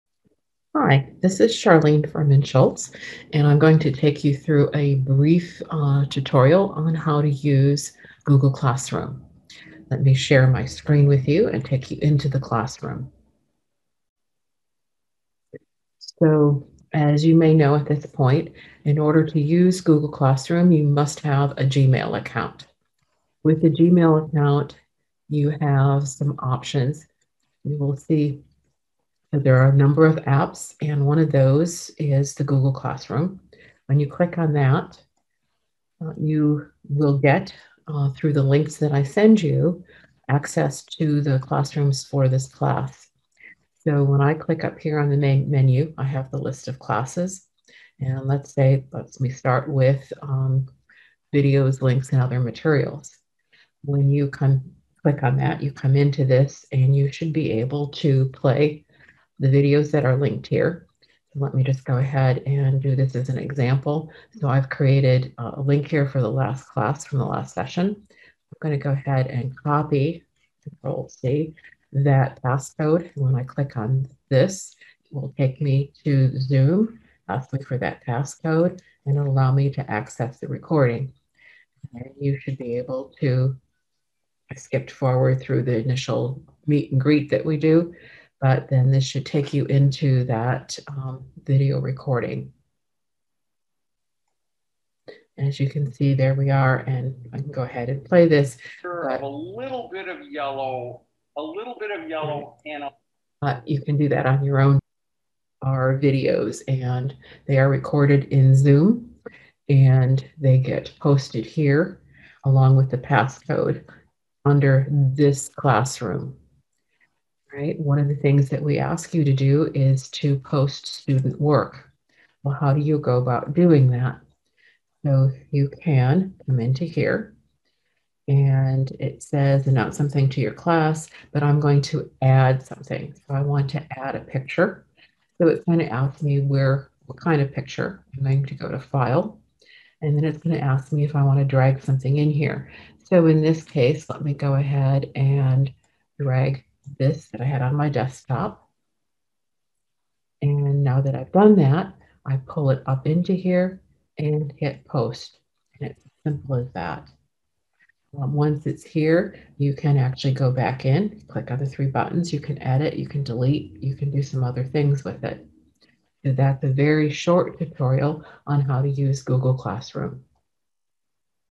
Using Google Classroom
A short video showing students how to access google classroom.